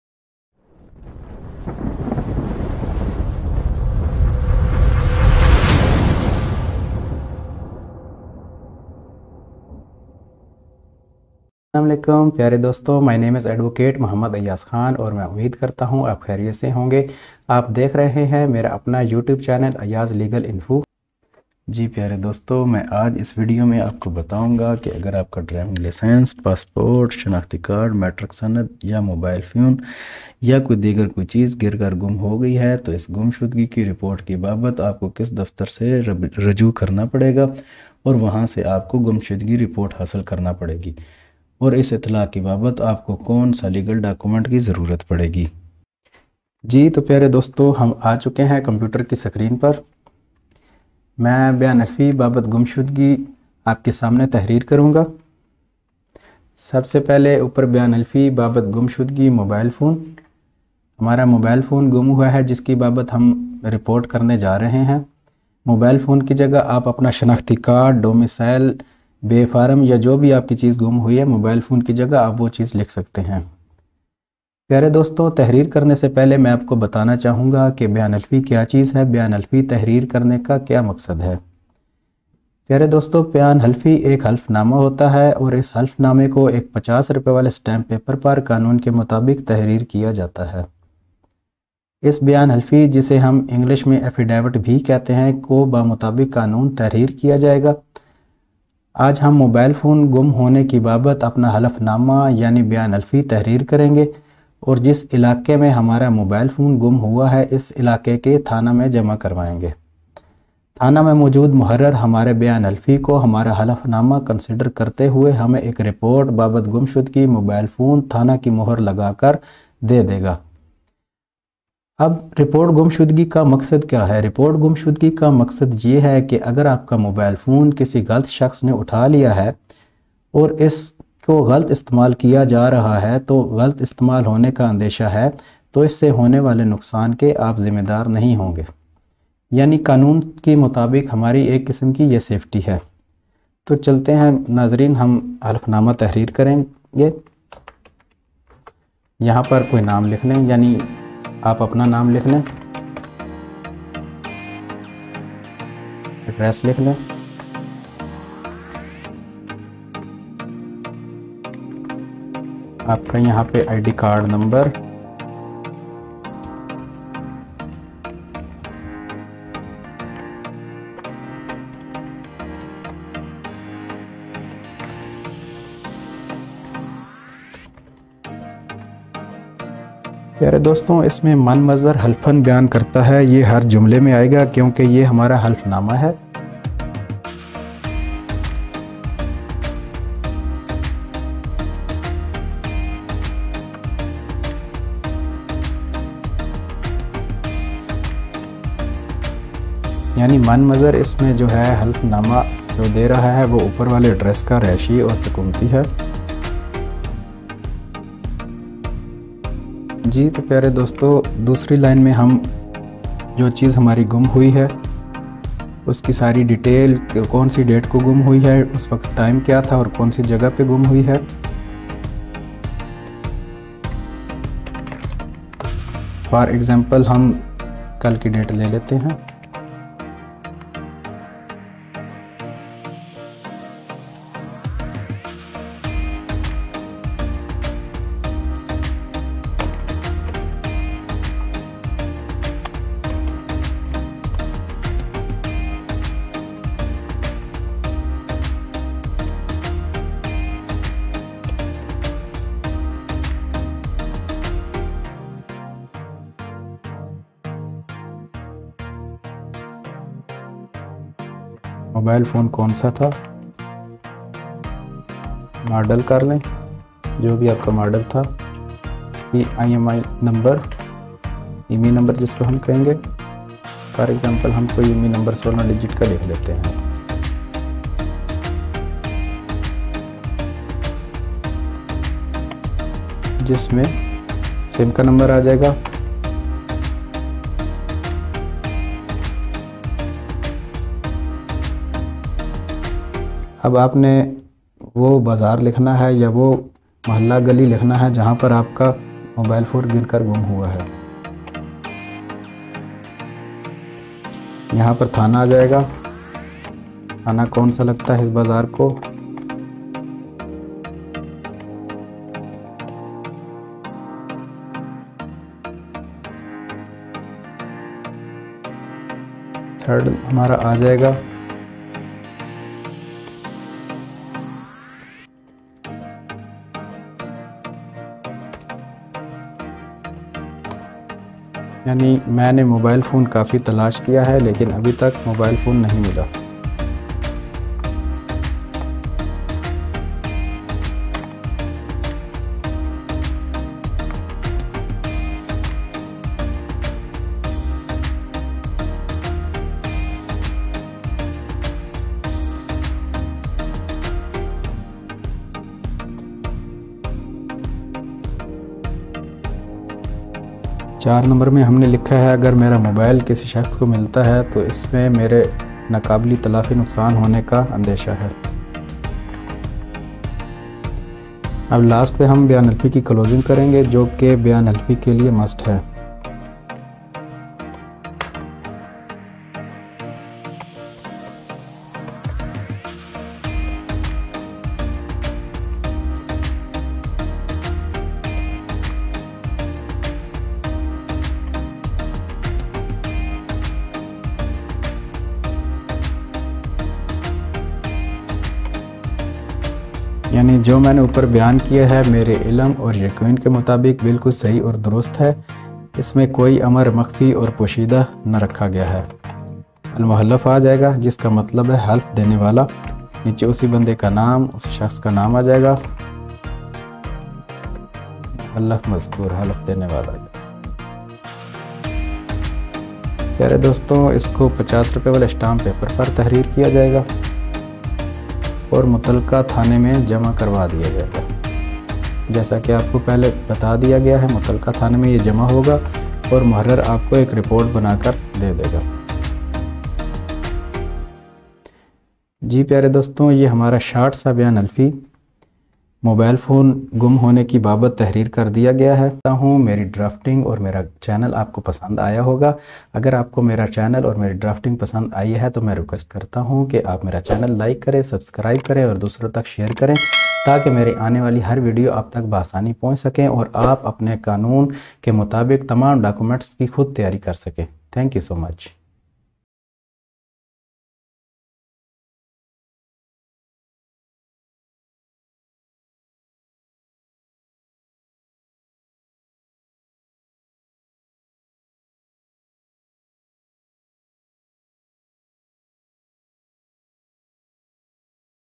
Lost Mobile Phone Report
Mobile Lost Affidavit - Report to police about loss of Mobile

In this video, I will describe that how we can get Police Report from Police Station regarding the lost of my Mobile Phone